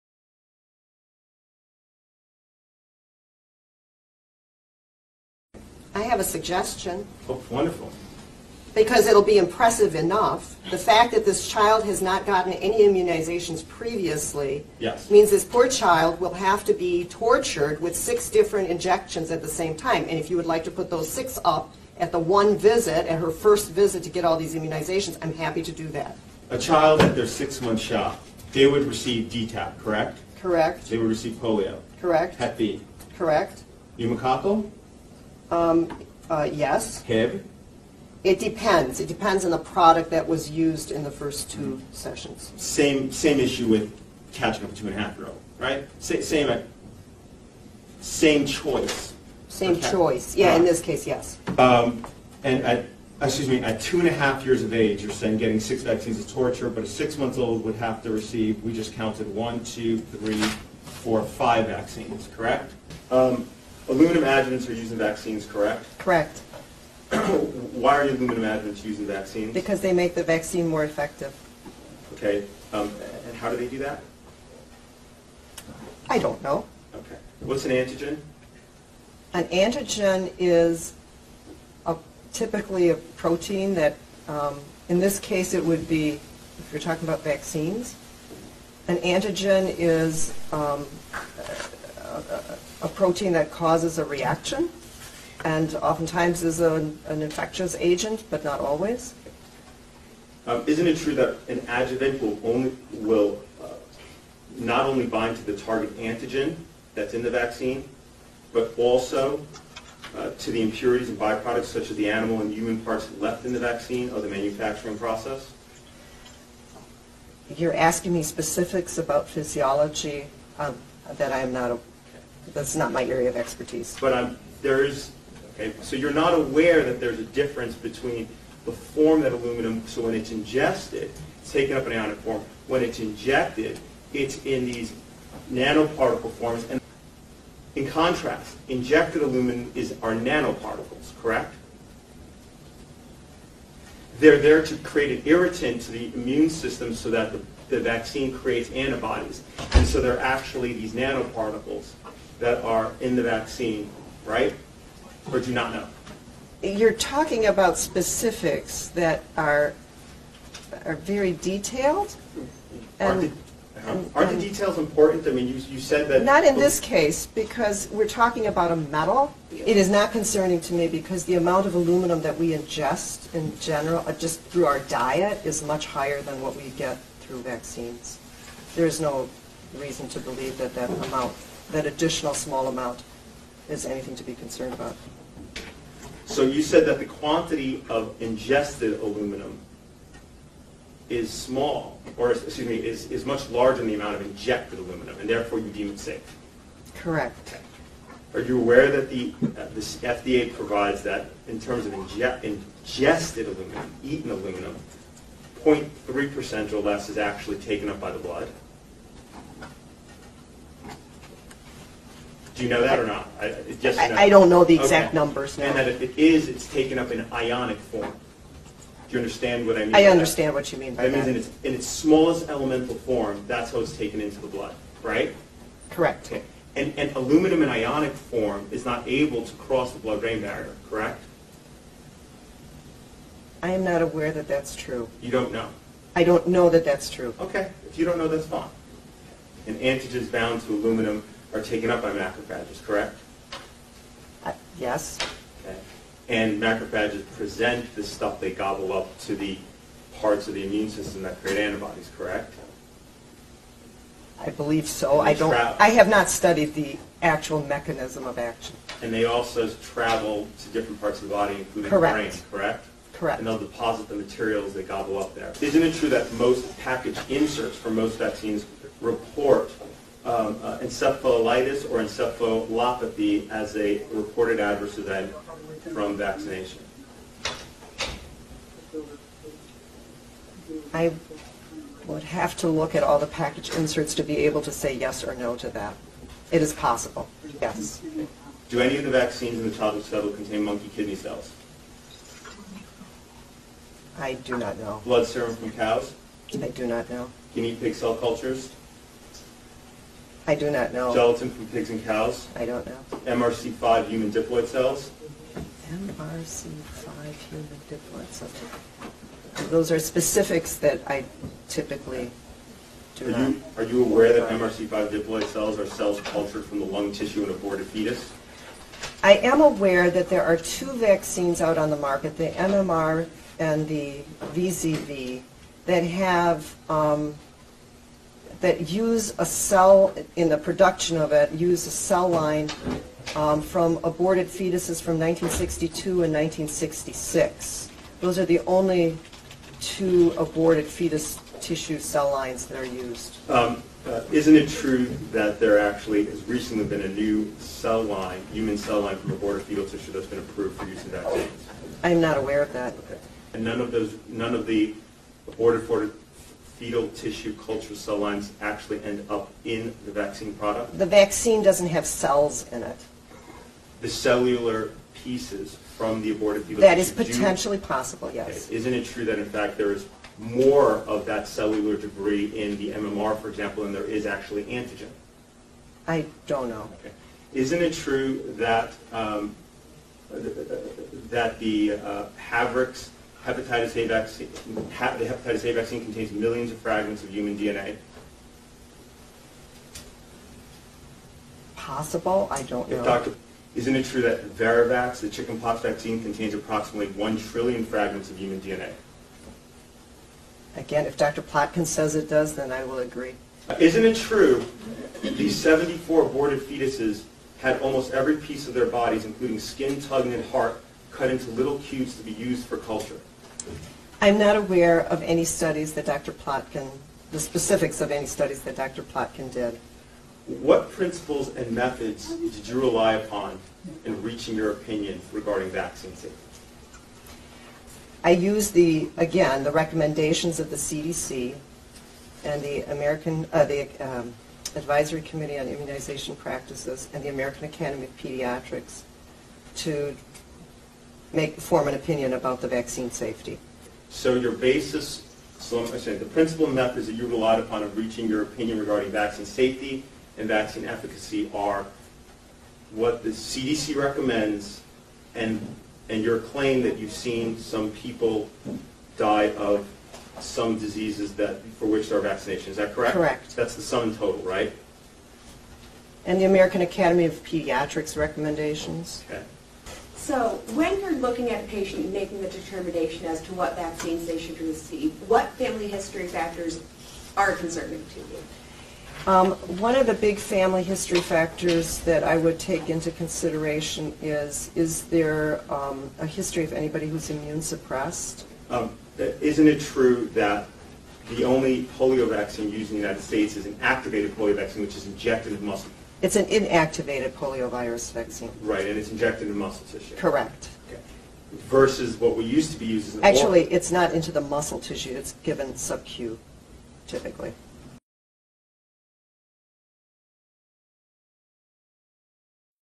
AMAZING TESTIMONY OF Dr Teresa Holtrop (preș. Academiei Americane de Pediatrie din Michigan)
Sunt curios câți dintre cei care recomandă vaccinurile cunosc ce recomandă? Sau câți dintre ei au citit un prospect de la un vaccin.

 Ar fi interesant dacă am copia întrebările și le-am adresa fiecare medicilor noștri dragi, cu tot respectul. Noi nu suntem specialiști, tocmai de aceea noi doar să-i întrebăm pe specialiști, cel puțin aceleași întrebări. De ce nu?